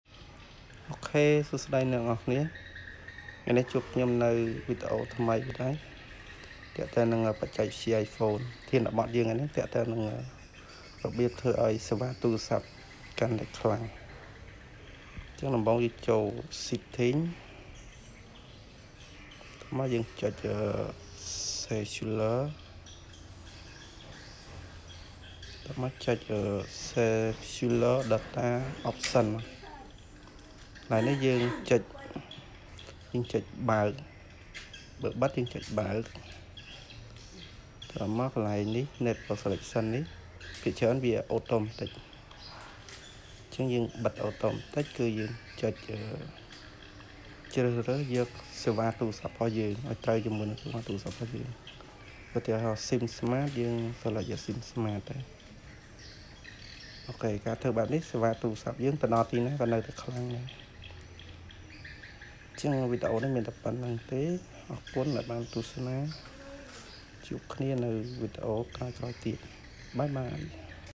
How to make Cellular strong on iPhone?របៀបធ្វើអោយសេវាទូរសព្ទ័ខ្លាំងទោះនៅទីណាក៍ដោយ!
បទបង្ហាញ ការធ្វើយអោយសេវាទូរសព្ទ័ខ្លាំង